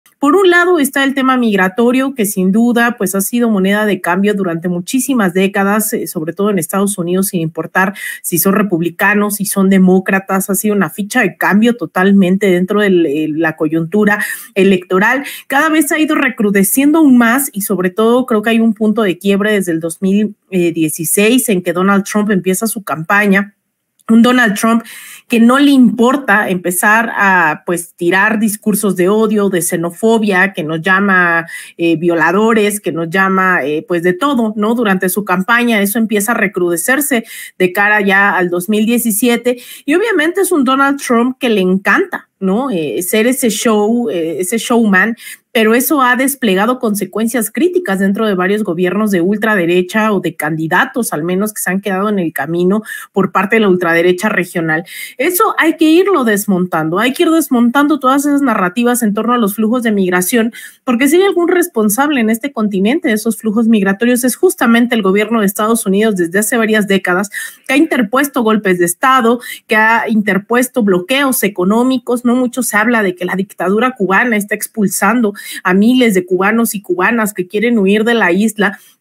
ES IMPORTANTE DESMANTELAR NARRATIVAS DE 0D!0 CONTRA FLUJOS DE MIGRACIÓN: ALINA DUARTE
El tema migratorio es moneda de cambio dentro de la coyuntura electoral y desde que llegó Trump en 2016 hay un quiebre pues con sus discursos de 0d10 y x3n0f0b14 en el que llama a los indocumentados v10l4d0r3s ha desplegado consecuencias críticas dentro de la derecha.
VICENTE SERRANO: Director de Sin Censura